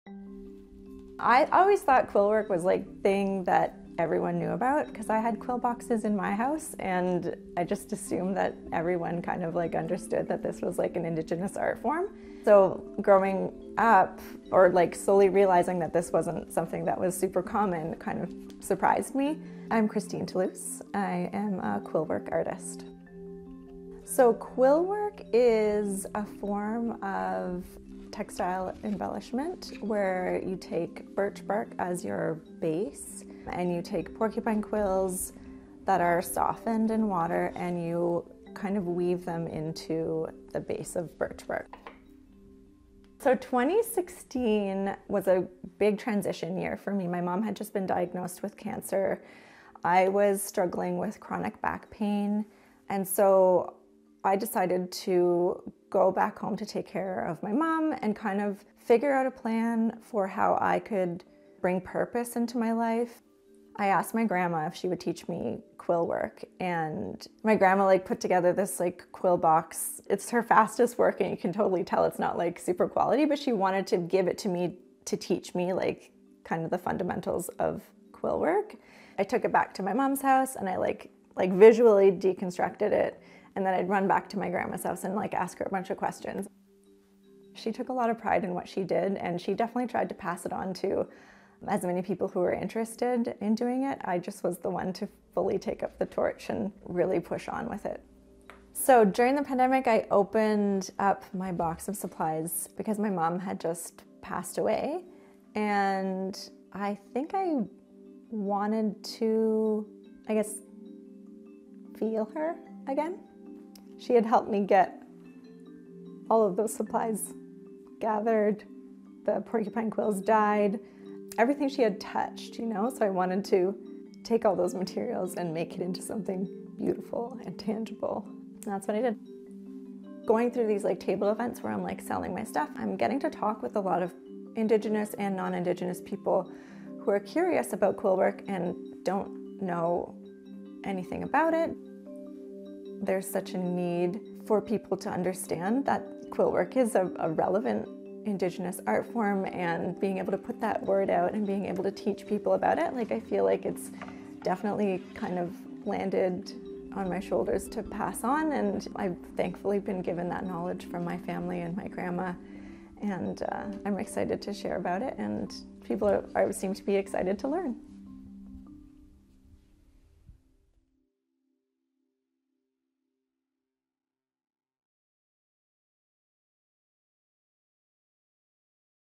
She makes art with porcupine quills and birch bark
Artist Christine Toulouse picked up the Indigenous practice of quillwork art during the pandemic. She shares how it helped her mourn her mother's loss — and why she's now teaching the art to others.

Read her story here cbc.ca/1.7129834